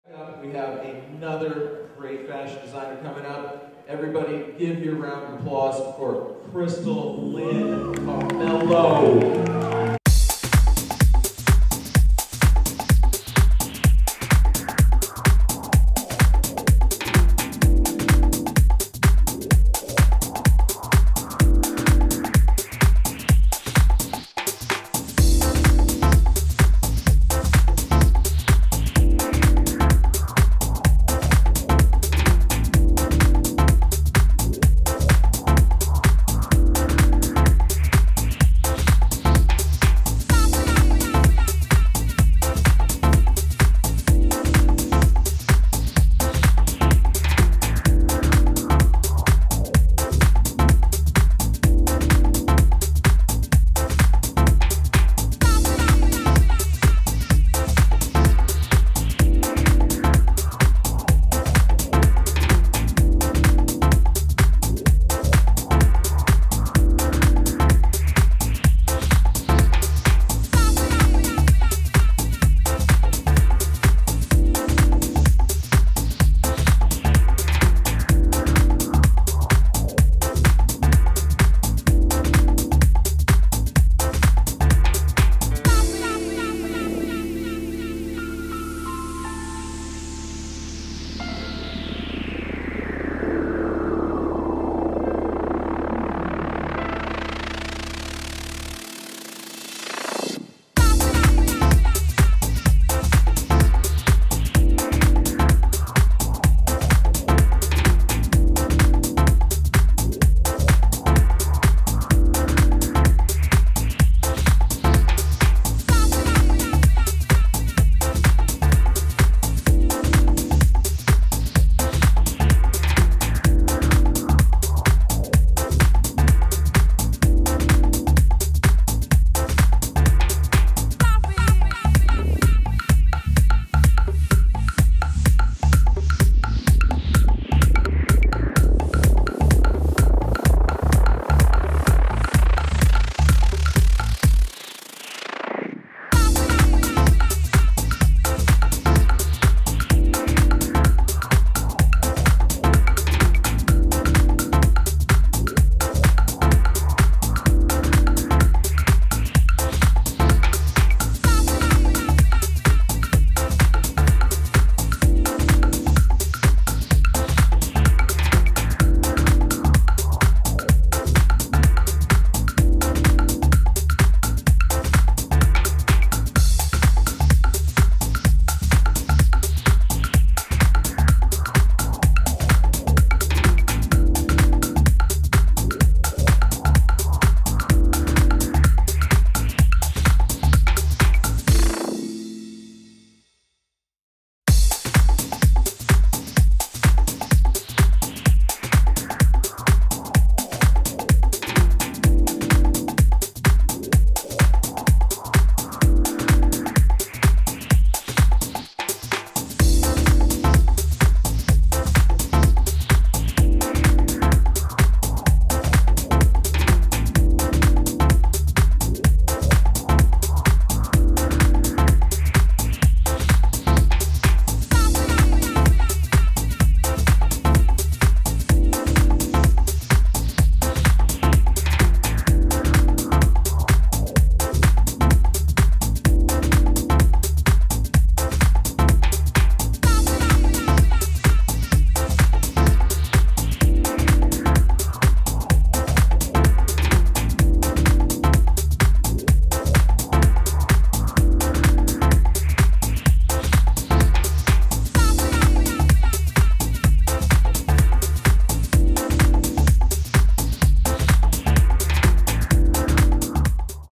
VIP Fashion Show 2012 - Crystal Carmelo.mov
Crystal Carmelo clothing line and model walk at the VIP Fashion Show 2012.